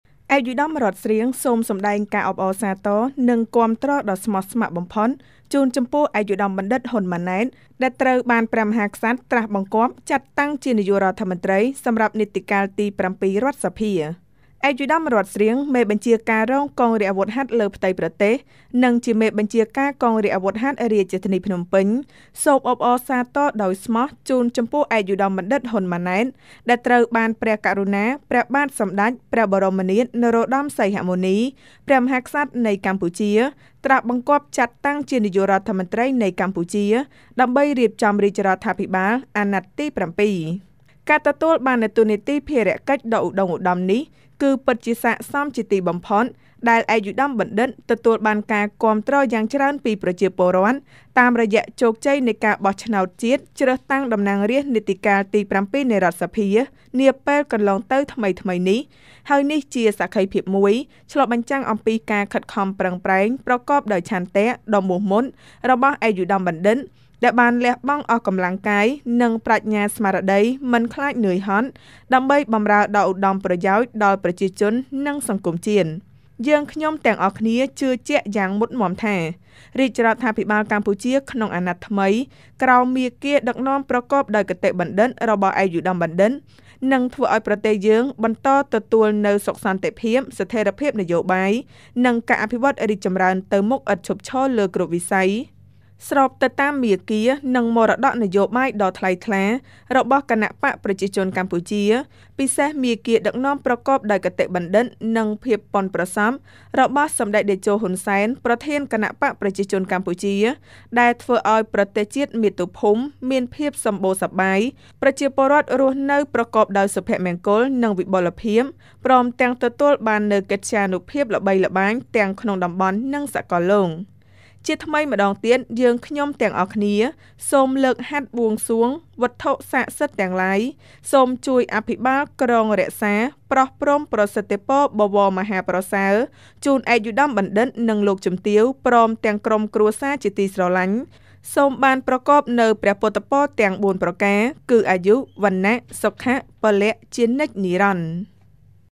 អបអរសាទរ និងគាំទ្រដ៏ស្មោះស្ម័គ្របំផុតជូនចំពោះ ឯកឧត្តមបណ្ឌិត ហ៊ុន ម៉ាណែត
ឯកឧត្តម រ័ត្ន ស្រ៊ាង សូមសម្តែងការអបអរសាទរ និងគាំទ្រដ៏ស្មោះស្ម័គ្របំផុតជូនចំពោះ ឯកឧត្តមបណ្ឌិត ហ៊ុន ម៉ាណែត ដែលត្រូវបានព្រះមហាក្សត្រ ត្រាស់បង្គាប់ចាត់តាំងជា នាយករដ្ឋមន្ត្រី សម្រាប់នីតិកាលទី៧ រដ្ឋសភា